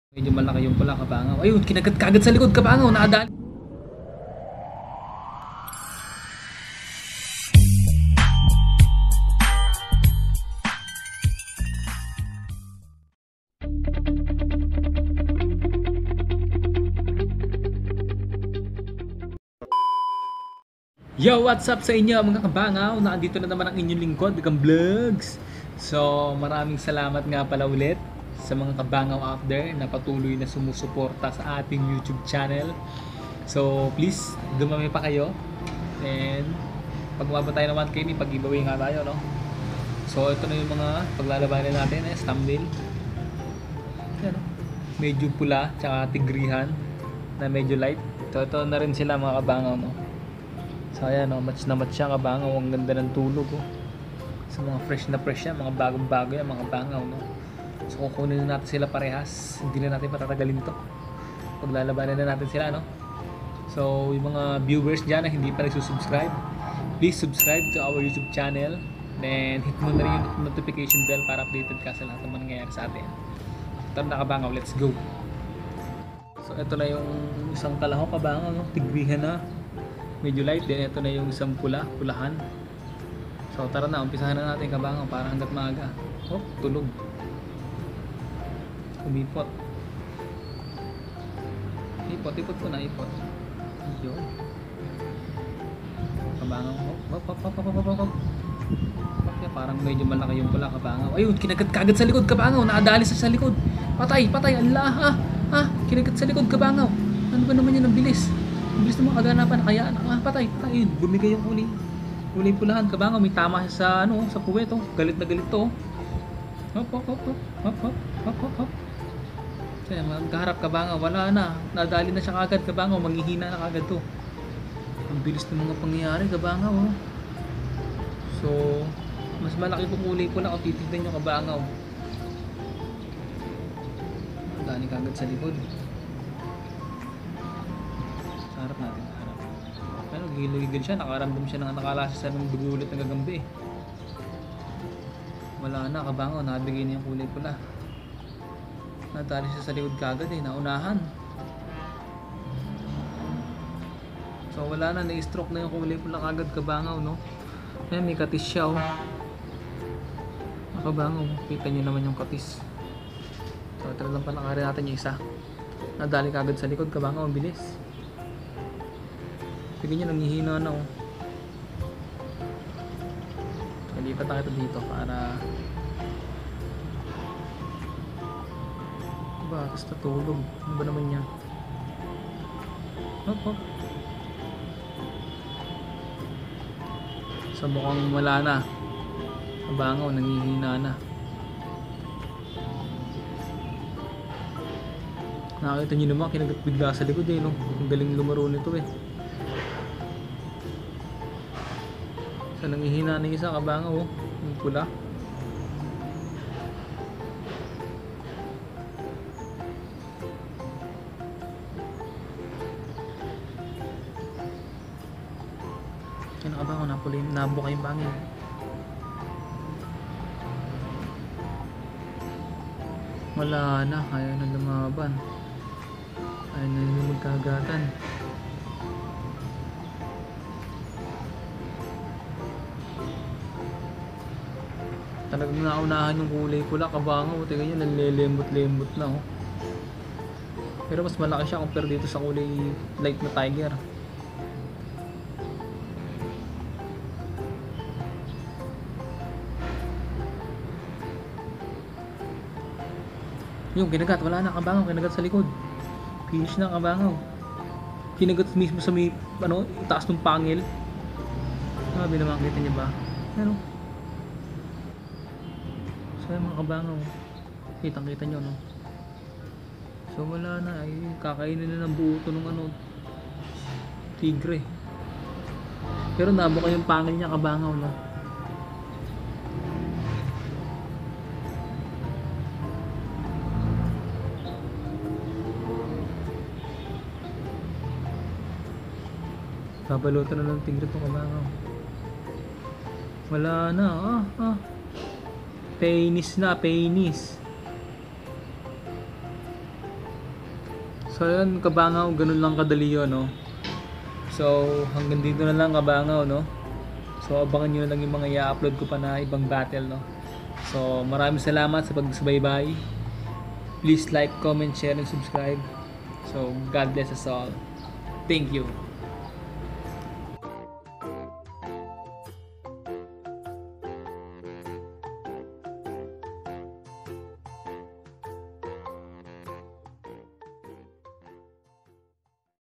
Spider fight | Speed lang | Masbate
Yow mga ka bangaw ako ang inyong lingkod gagamvlogs 2019. Panuorin at tangkilikin ang larong ating kinahiligan mula noon hangang ngayon. Please support all filipino YouTube content creator.

Gagamba
90'kids